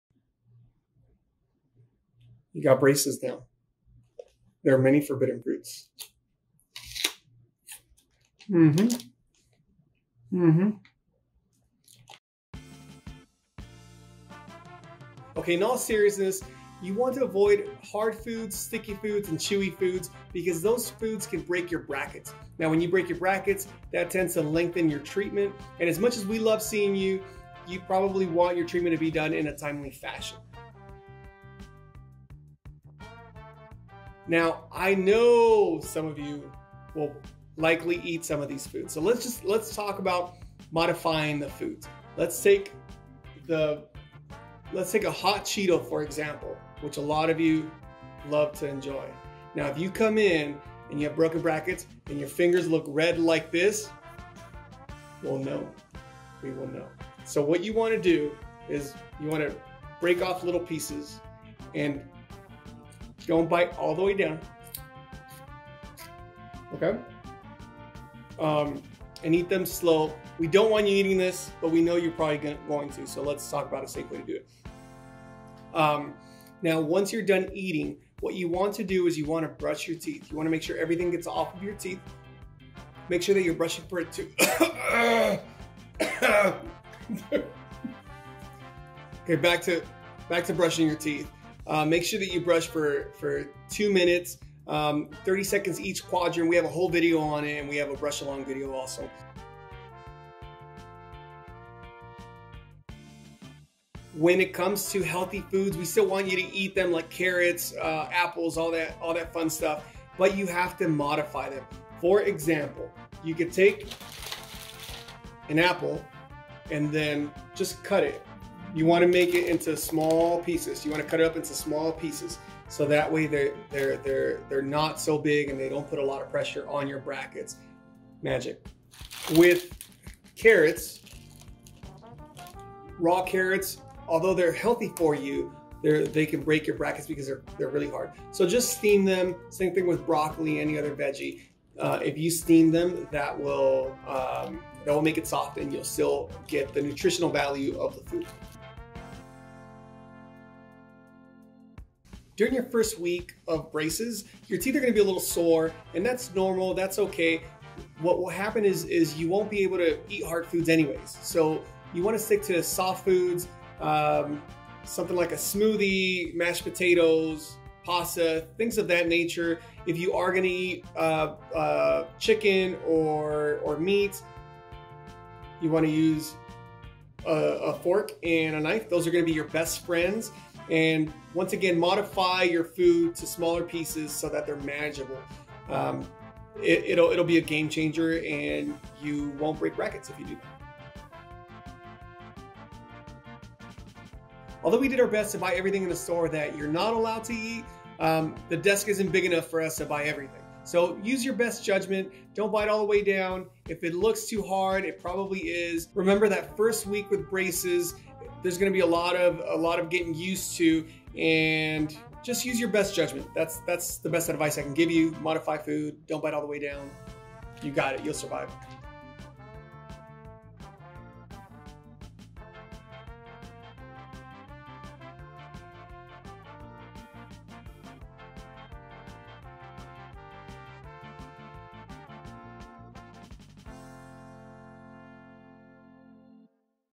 Foods to Avoid with Braces Tips & Tricks By Smile Squad LV
Dr. Martinez is here to remind you about foods to avoid with braces on. You will also find tips & tricks like, ways to modify and slow your food bites to adjust to eating with braces on.